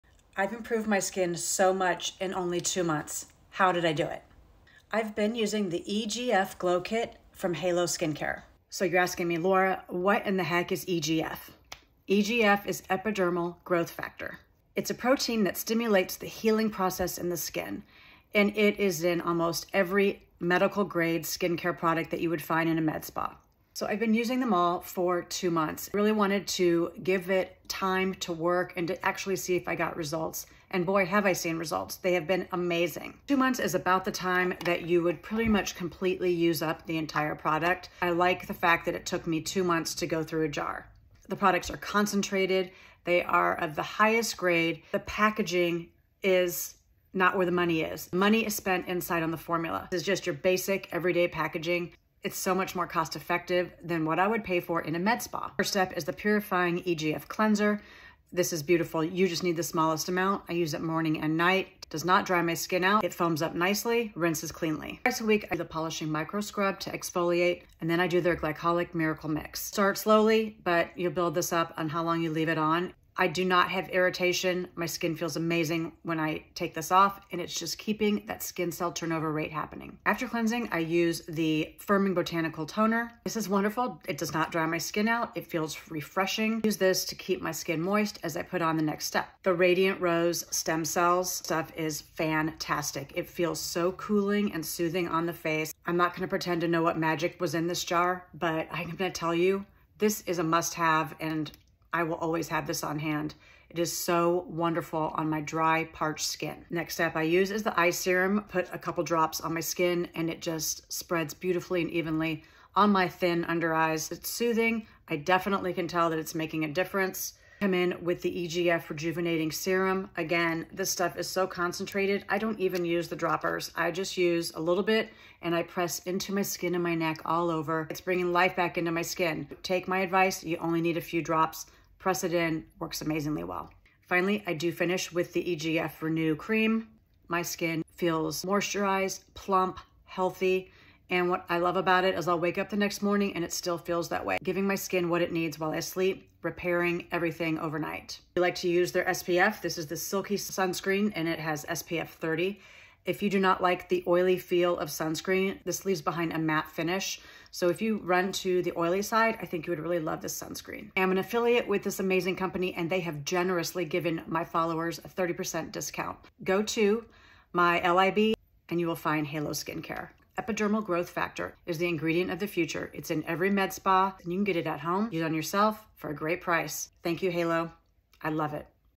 Two Months of Consistent Skincare Results!
I used ​⁠Heilo Skincare for TWO MONTHS and my results were EXCELLENT. EGF (Epidermal Growth Factor) is the ingredient found in many high-end med spa lines.